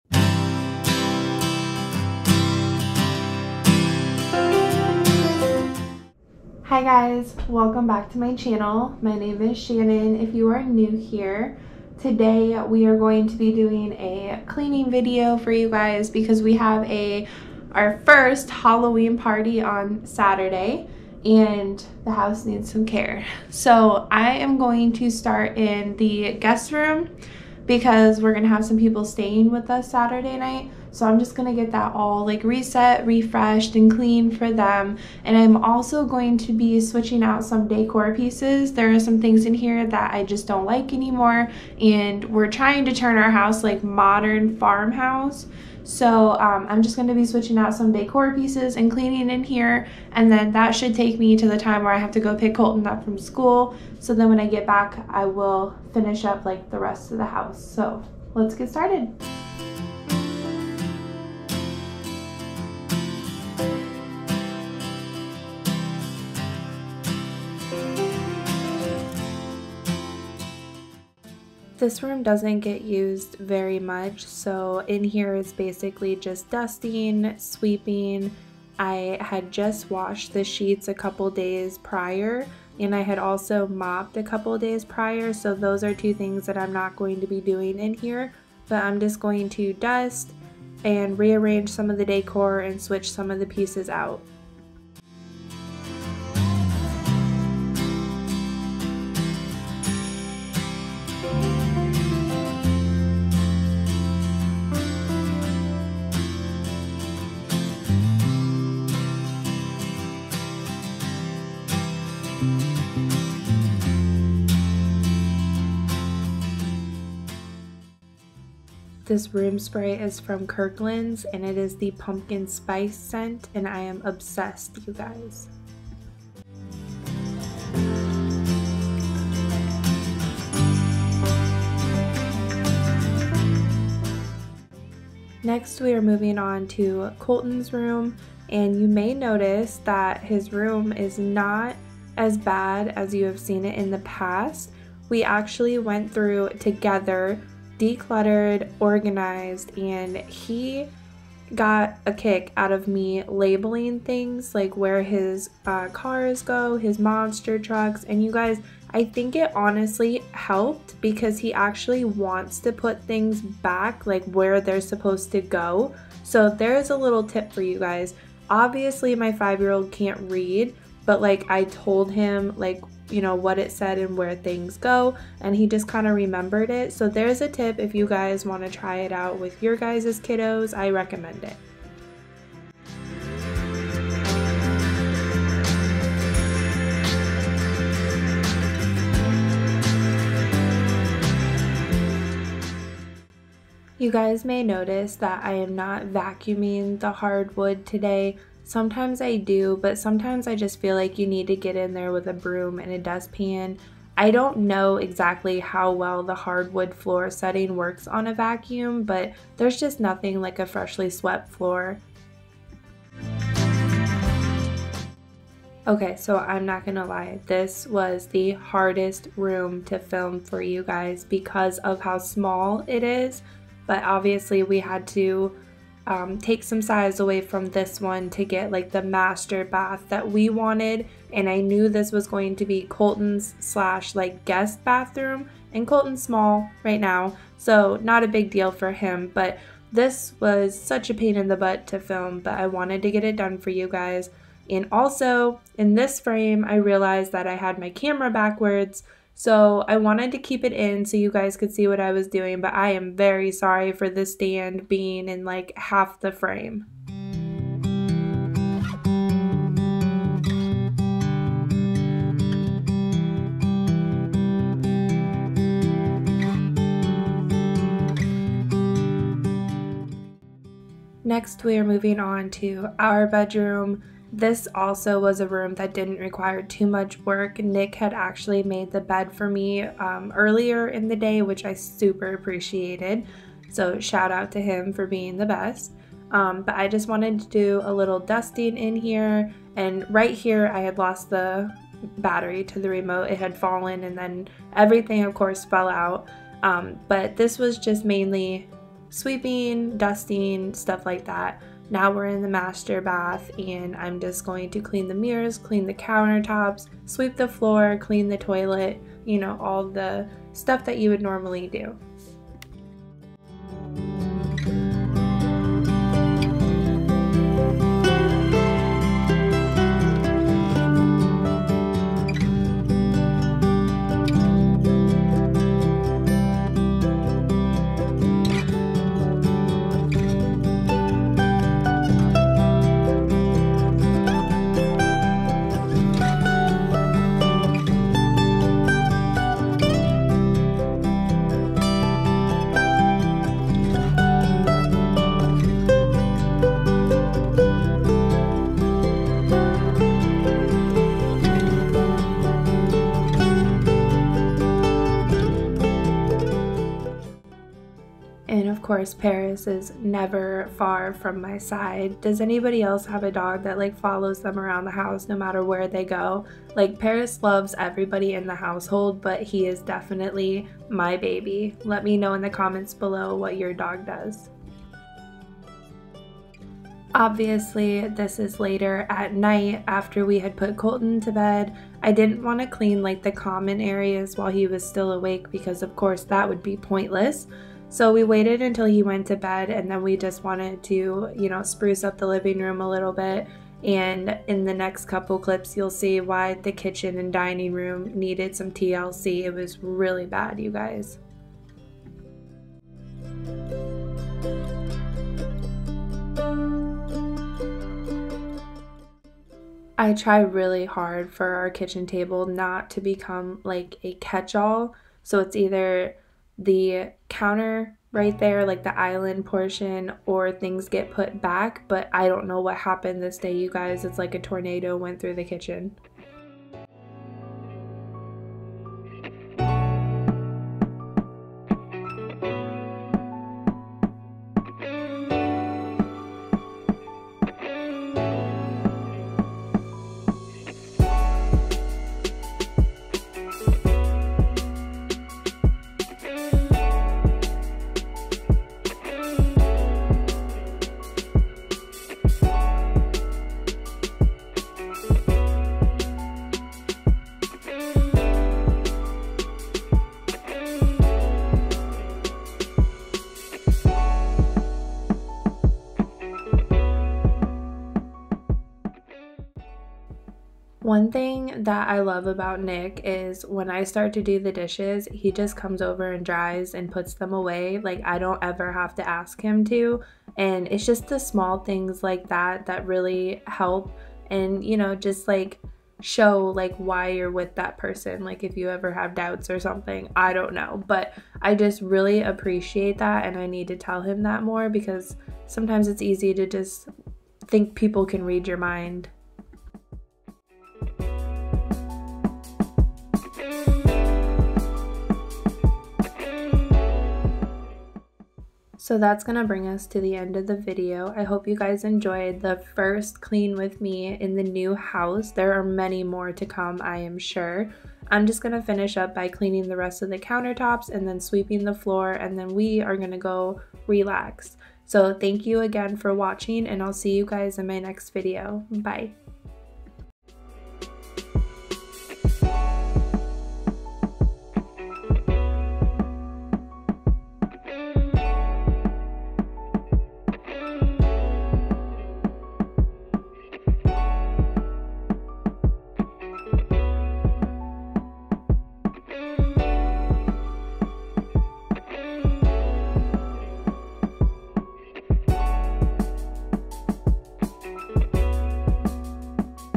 First Ever Clean With Me in the New House | Cleaning Motivation
Well, it's Tuesday again! Here is a little bit of cleaning motivation to help you get through the day! First clean with me in the New house. This was so fun to make, because it's a whole new learning experience! New places, new spots, new things to do and more! Hope you guys enjoy!